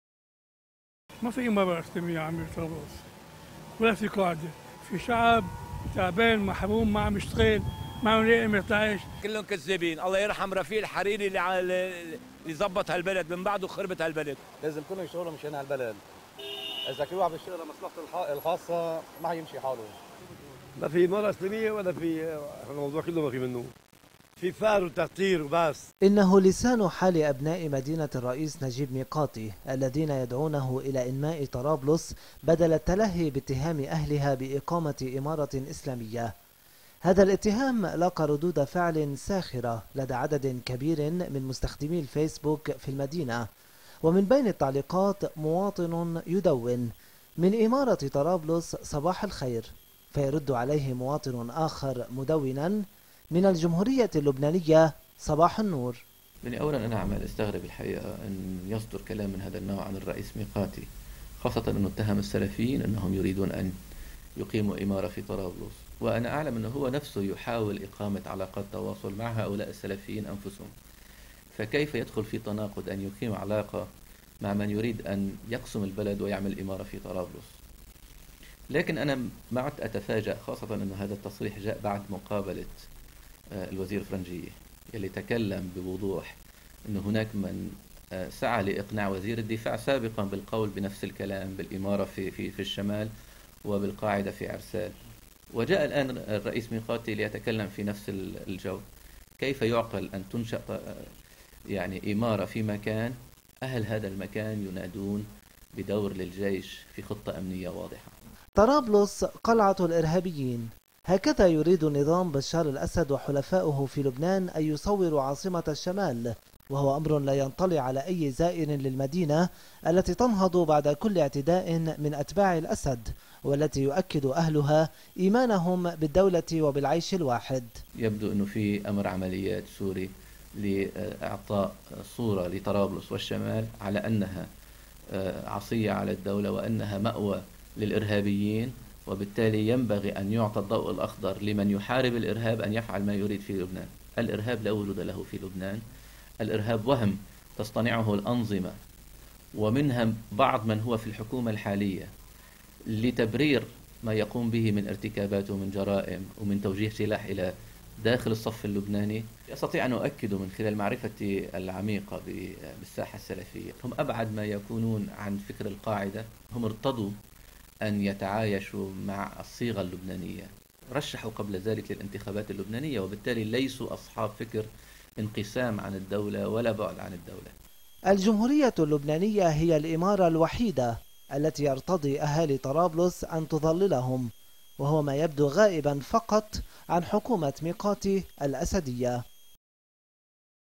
طرابلس ترفض كلام ميقاتي عن إمارة إسلامية في المدينة
يلاقي كلام رئيس مجلس الوزراء نجيب ميقاتي عن اقامة امارة اسلامية في طرابلس استهجانا كبيرا لدى أبناء المدينة الذين يؤكدون أنهم يريدون العيش بكرامة تحت سقف الدولة اللبنانية.
 تقرير ربيع شنطف